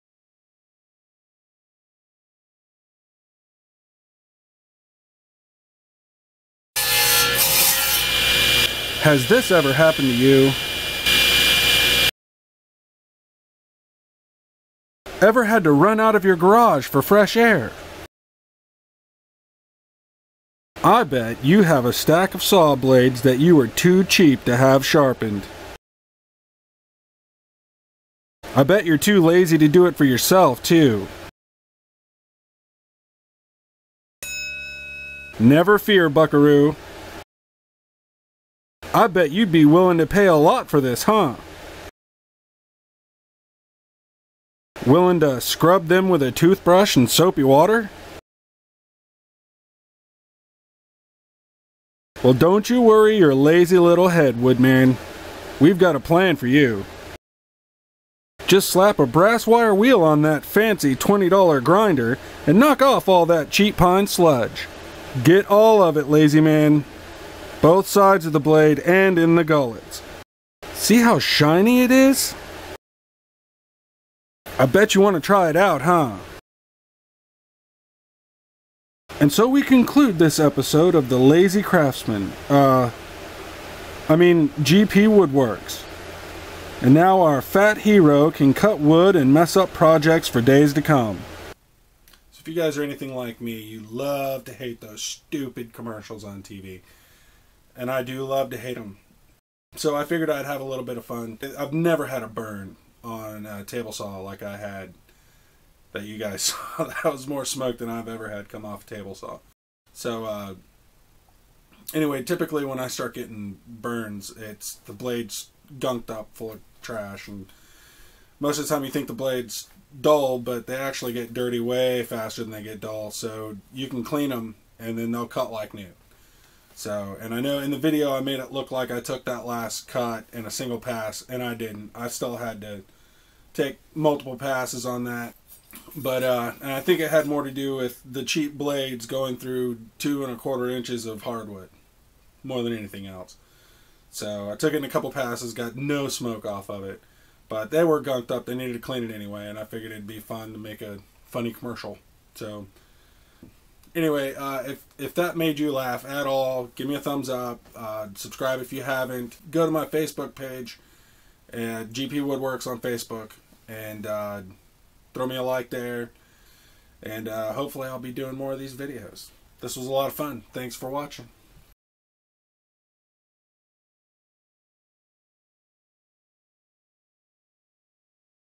GPW 4 - Stoopid Commercial Tip
A tip for saw blades in the form of a stupid TV commercial.